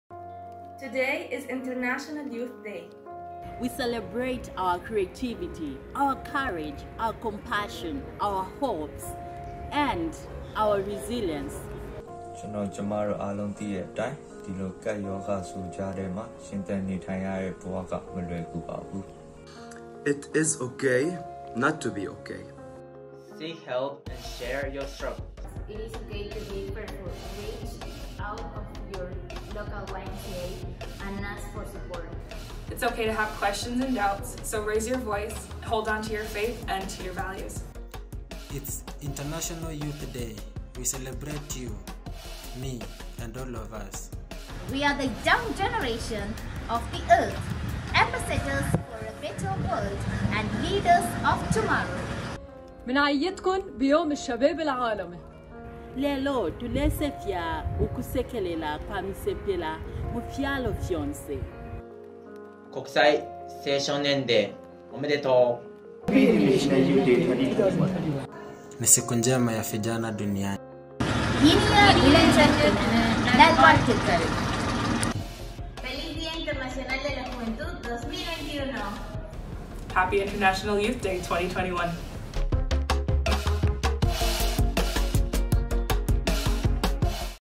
International YouthDay 2021
Today is International Youth Day.

Today we celebrate our creativity, our courage, our compassion, our hopes,  and our resilience.

We know that life is not easy at the moment. The pandemic took a lot from us.

It is ok not to be ok. Seek help and share your struggles.
It is ok to be fearful. Reach out to your local YMCA and ask for support.
It is ok to have questions and doubts. Raise your voice and hold on to your faith and to your values.

Today is International Youth Day and we celebrate you, me, and all of us. We are the young generation of the earth, ambassadors for a better world and leaders of tomorrow.

Happy International Youth Day 2021!